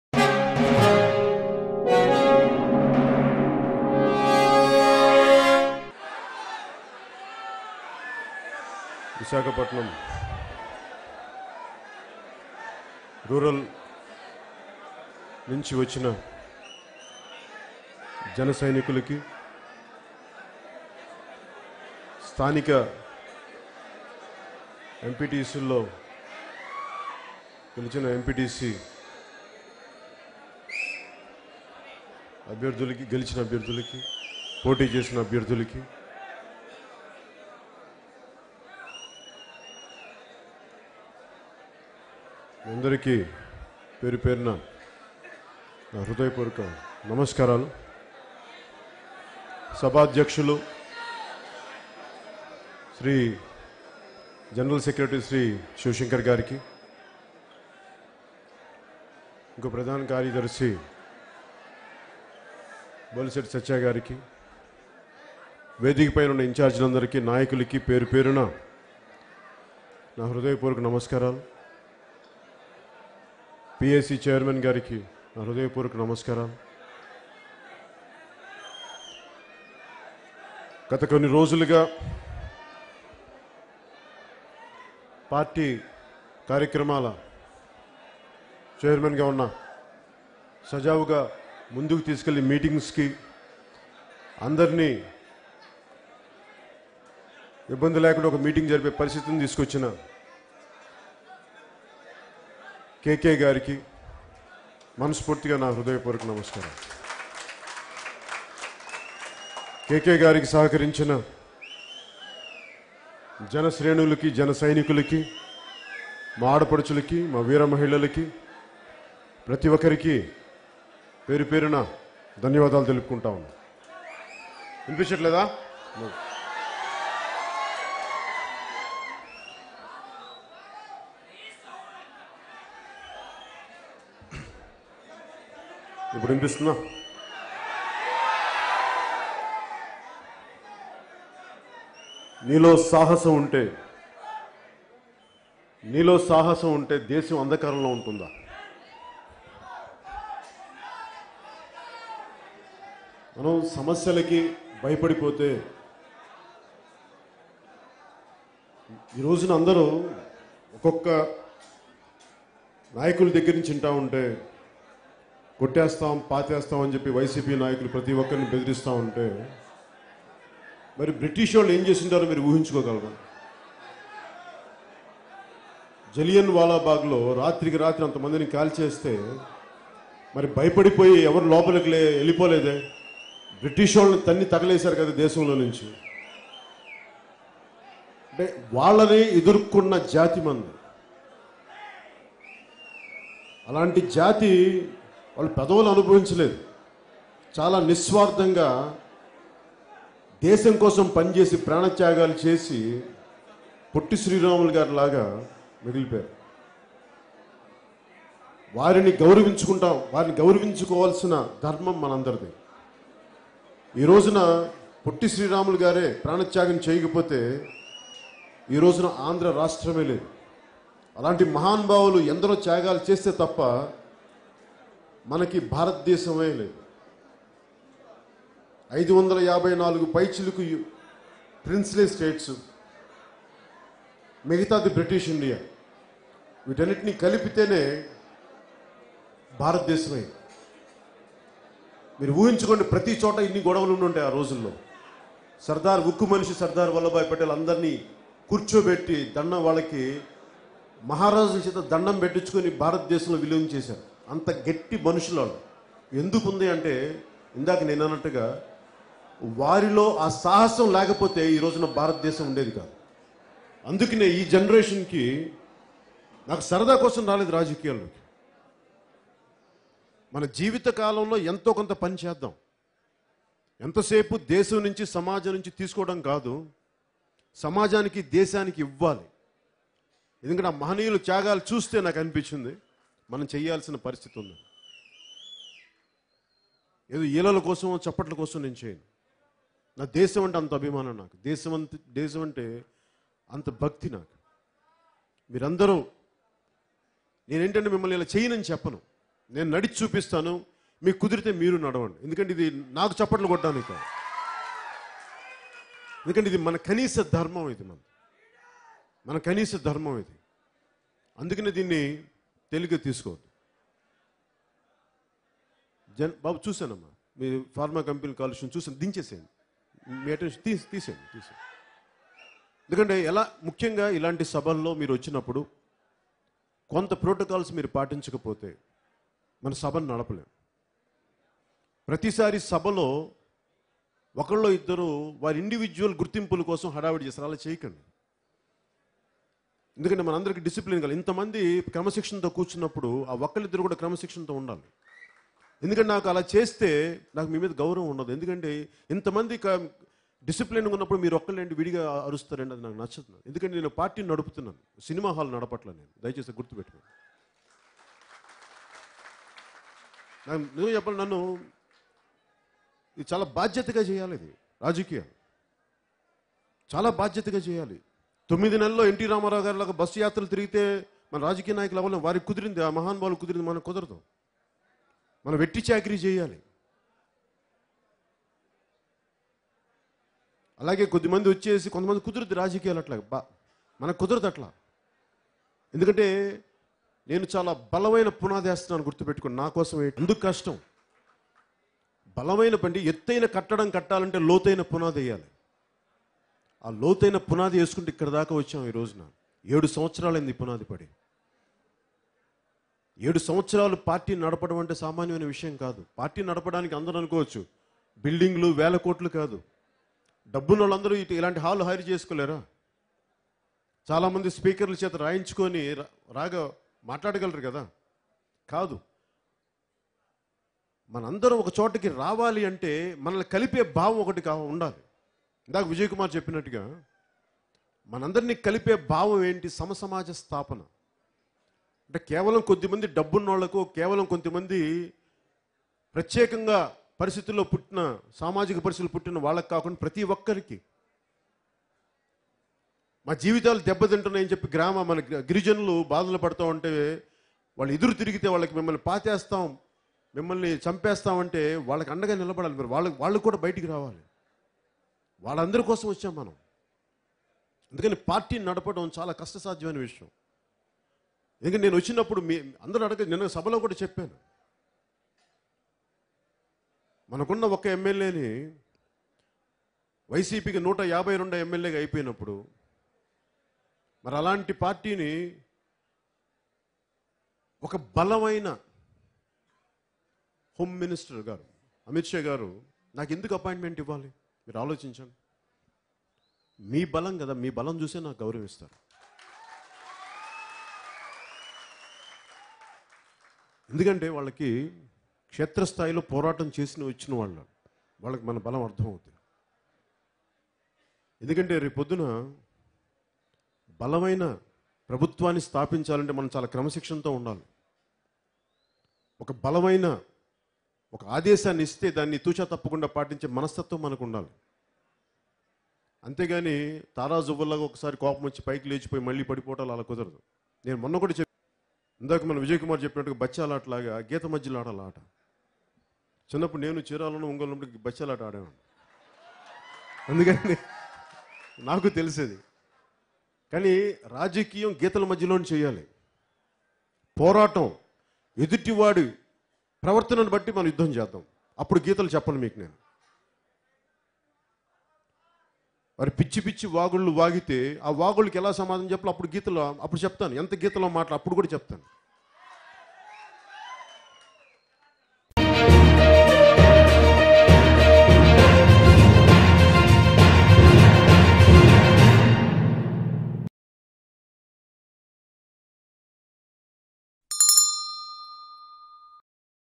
మీ సొంత గుర్తింపు కోసం చేస్తున్నారు జన సైనికులు మీద పవన్ ఫైర్ | Pawan Kalyan Fre on Janasainik#KNtv
మీ సొంత గుర్తింపు కోసం చేస్తున్నారు జన సైనికులు మీద పవన్ ఫైర్ | Pawan Kalyan Fre on Janasainik